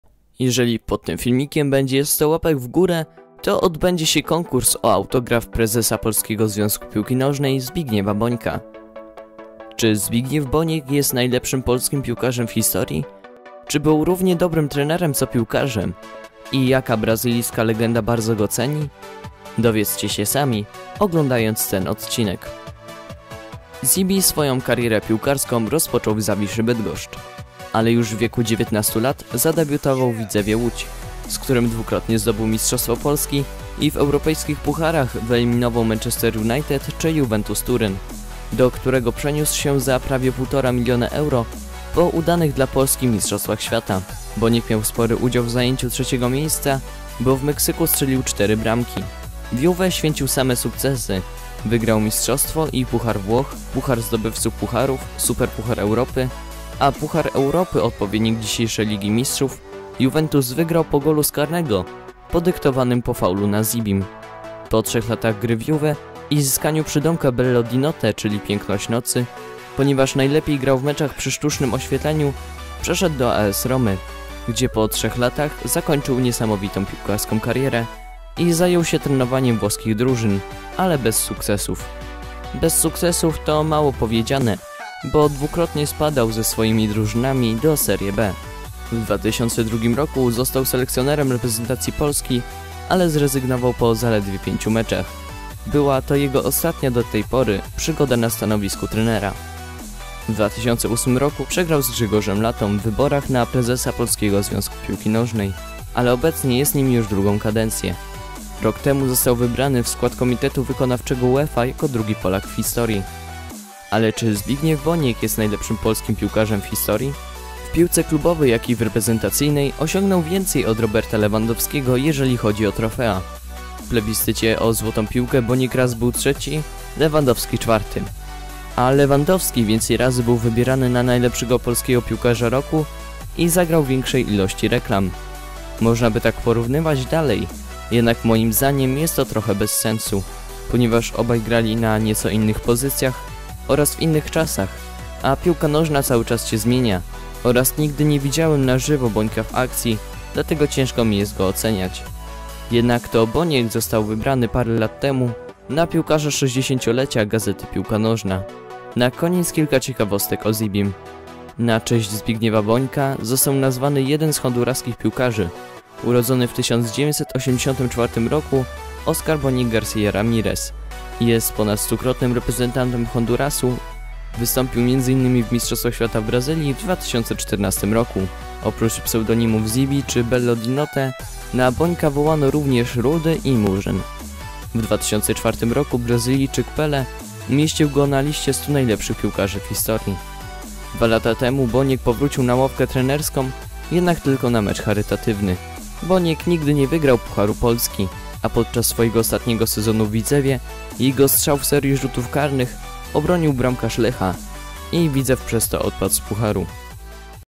Boniek najlepszym polskim piłkarzem?
Czy obecny prezes Polskiego Związku Piłki Nożnej - Zbigniew Boniek jest najlepszym polskim piłkarzem w historii?
Zachęcam do zostawienia łapki w górę, jeżeli będzie będzie ich 100 to odbędzie się konkurs o autograf Zibiego.